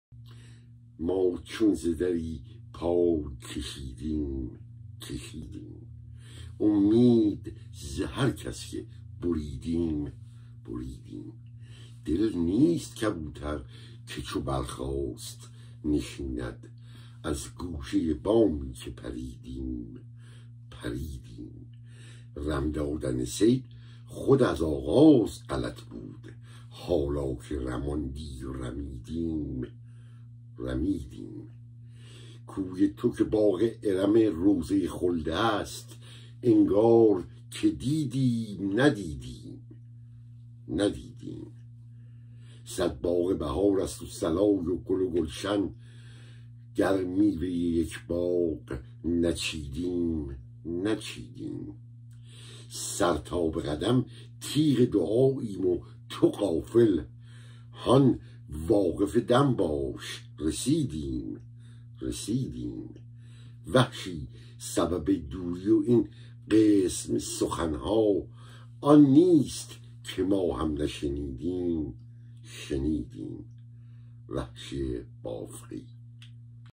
وحشی بافقی : ما چون ز دری پای کشیدیم ٕکشیدیم ( خوانش : سهیل یغمایی )
ما چون ز دری پای کشیدیم کشیدیم
امید ز هر کس که بریدیم ، بریدیم
دل نیست کبوتر که چو برخاست نشیند
از گوشهٔ بامی که پریدیم ، پریدیم
رم دادن صید خود از آغاز غلط بود
حالا که رماندی و رمیدیم ، رمیدیم
کوی تو که باغ ارم روضهٔ خلد است
انگار که دیدیم ندیدیم، ندیدیم
سد باغ بهار است و صلای گل و گلشن
گر میوهٔ یک باغ نچیدیم ، نچیدیم
سرتا به قدم تیغ دعاییم و تو غافل
هان واقف دم باش رسیدیم، رسیدیم
وحشی سبب دوری و این قسم سخن‌ها
آن نیست که ما هم نشنیدیم ، شنیدیم